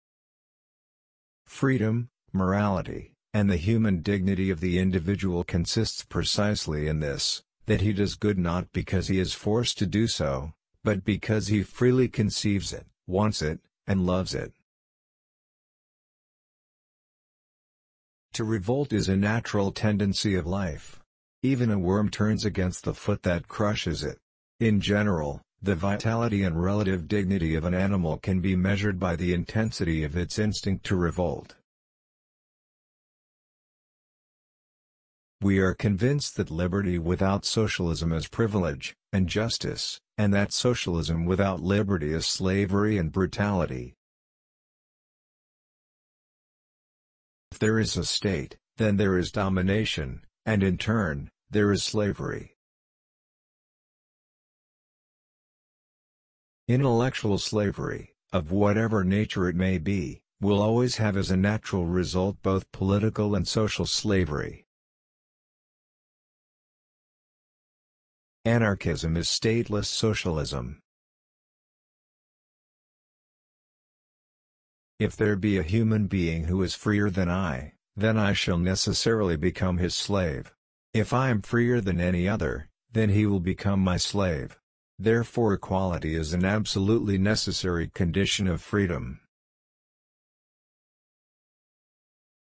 7 Quotes from MIKHAIL BAKUNIN that are Worth Listening To! Anarchism
Mikhail Alexandrovich Bakunin was a Russian revolutionary anarchist, socialist and founder of collectivist anarchism. He is considered among the most influential figures of anarchism and a major founder of the revolutionary socialist and social anarchist tradition. Bakunin's prestige as a revolutionary also made him one of the most famous ideologues in Europe, gaining substantial influence among radicals throughout Russia and Europe.
Mikhail Alexandrovich Bakunin adalah seorang politikus Rusia. Bakunin adalah salah satu dari pemikir anarkis terbaik. Bahkan banyak yang menyebut bahwa ia adalah salah satu "pendiri gerakan anarkisme". Bakunin merupakan seorang tokoh anarkis yang mempunyai energi revolusi yang dahsyat.